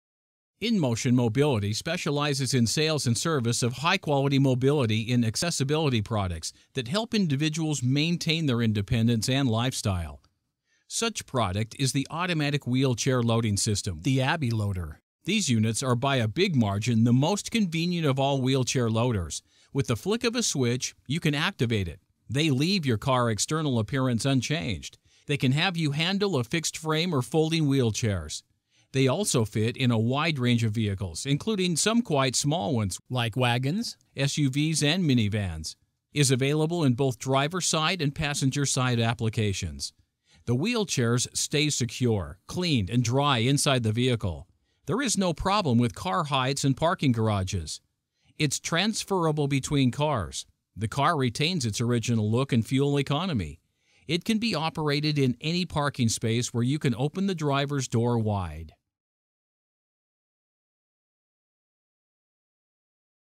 ABILOADER at Motion Mobility
The AbiLoader-FX is a robotic arm, which delivers Fixed-Frame wheelchairs from the luggage compartment of the vehicle to the front or side door of this vehicle.
The AbiLoader may be used in cars with a high rear door, vertical or inclined, such as estate cars, station wagons, minivans, MPV, SUV, off-roaders or 4WD.

The mechanism is mechanically adjustable to allow it to fit in a wide variety of vehicles. Once set it remains correct for the life of the device.
 The electrical operation is simple, requiring no electronics or calibration, for ease of installation and maintenance.

Chair unloading is driven by 2 industry-standard, highly reliable electrical actuators driven from the car electrical system. A third actuator, mounted on the main base, is used to open the boot, where this is not already electrically controlled.